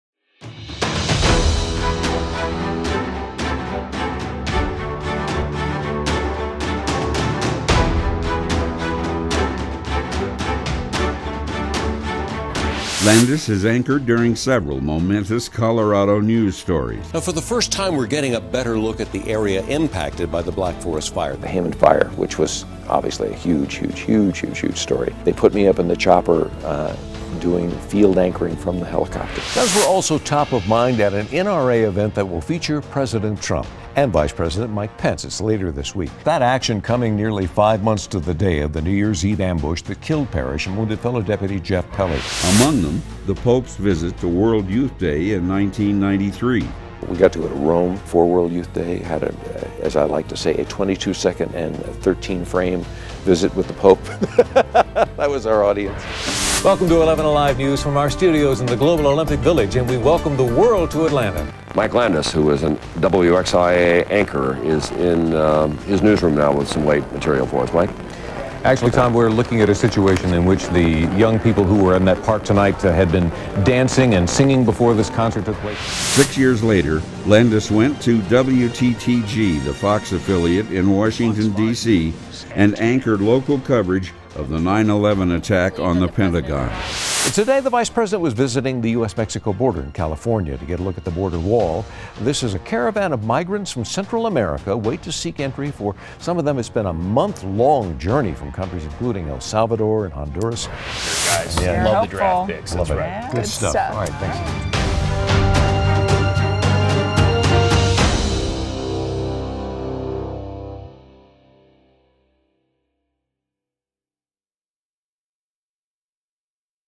MIKE LANDESS - TV NEWS ANCHOR/DIGITAL JOURNALIST
Mike Landess is a TV News Anchor and Digital Journalist with more than 50 years of broadcast experience. His social numbers have boosted him to the top 30 out 170 talent in the Denver-TV market. He's won 24+ Emmy Awards (several for 'Best Anchor) and 5 Edward R. Murrow Awards.